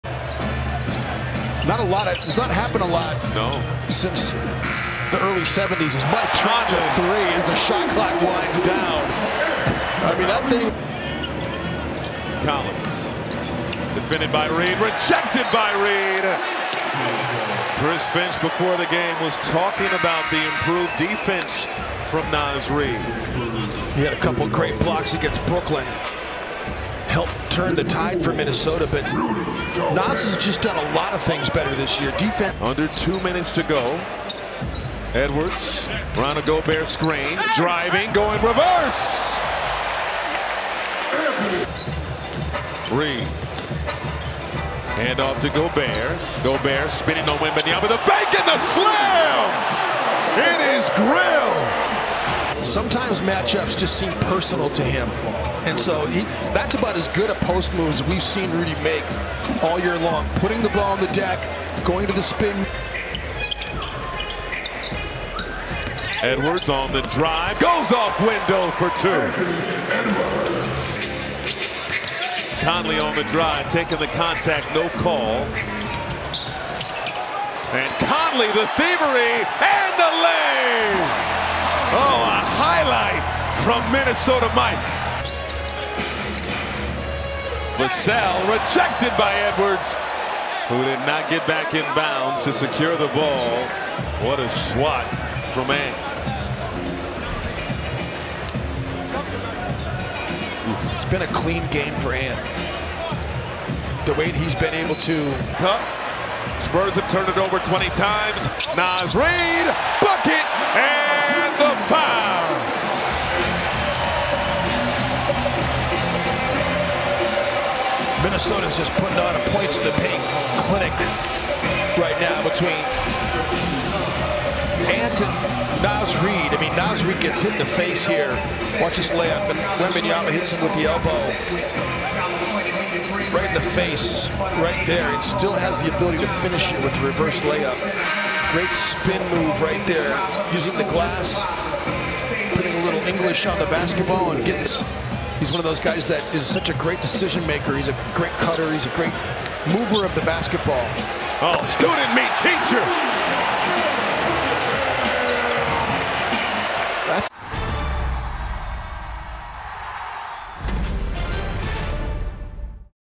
HIGHLIGHTS: Anthony Edwards puts on show vs. San Antonio
Highlights from the Timberwolves win over the San Antonio Spurs.